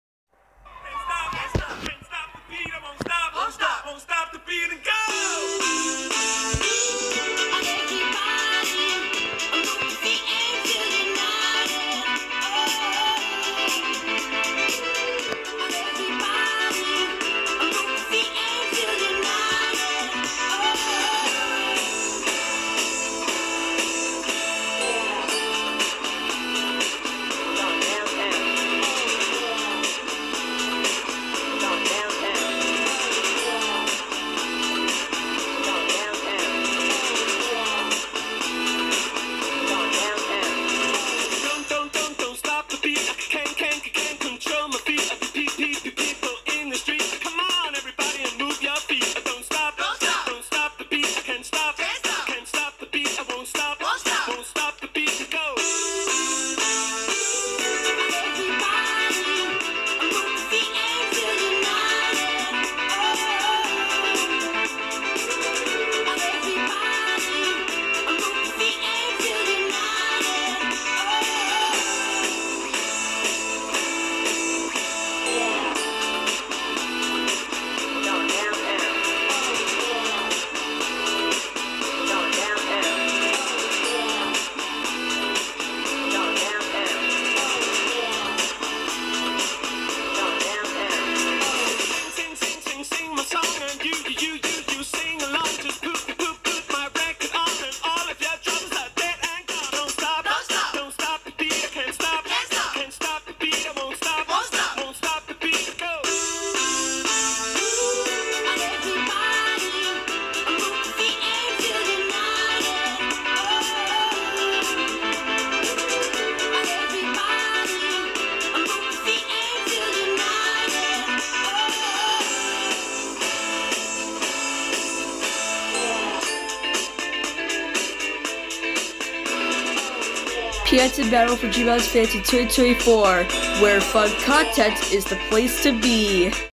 PABFDIF2024 New Intro (Don't Block This!) (Reupload) (Third Update)
Note: This took me 3 months to make this new intro for 2024 with new Obsession Squad Members.
Note: I don't want this new intro Partially blocked, This is just a masterpiece.
Note: I Updated It Because Tiny Miny Has Been Added And Is The New Obsession Squad Member For May 2024